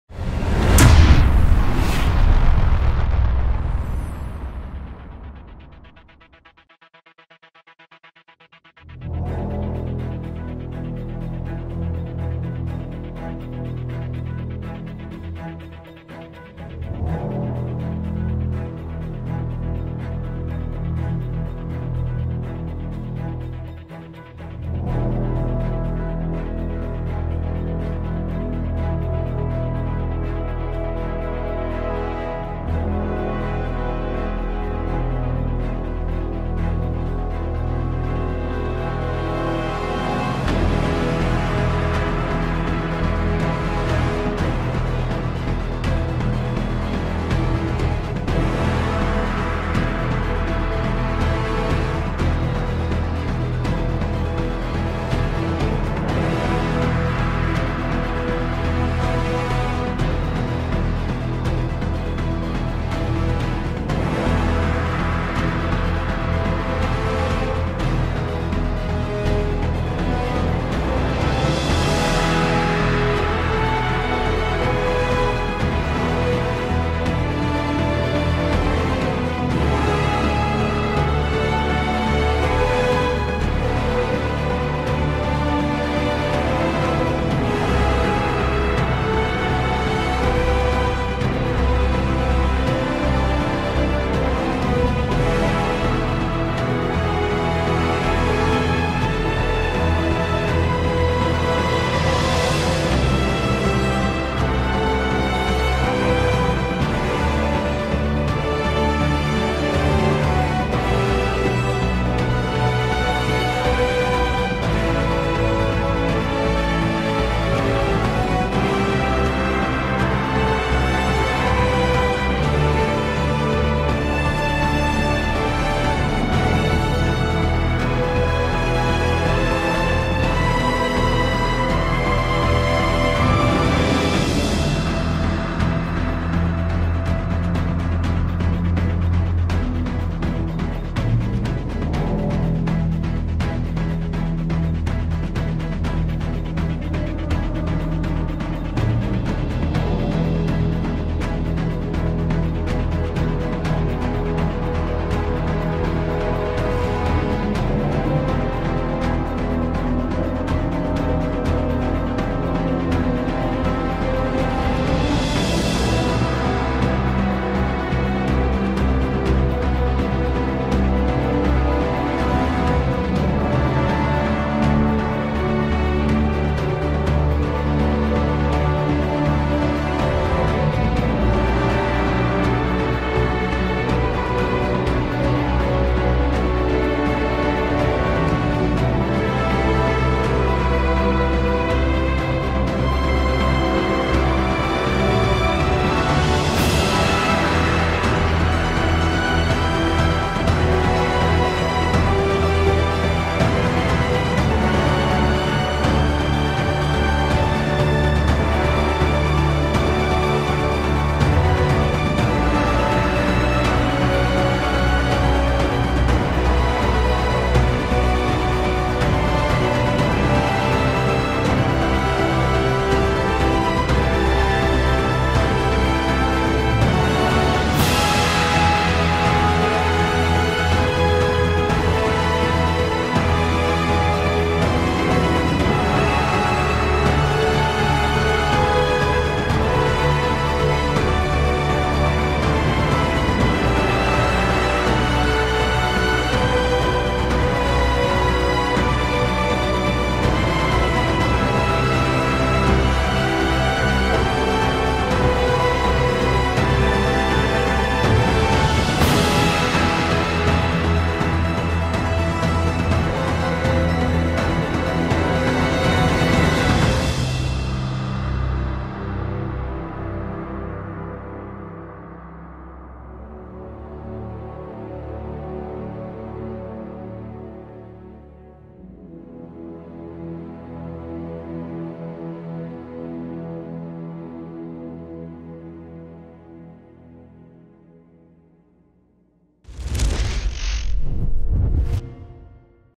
Speedpaint - Flaming Dragon [Fan Art]
Watchout for the fire!
Li'l fan art for LuxxArt on Twitter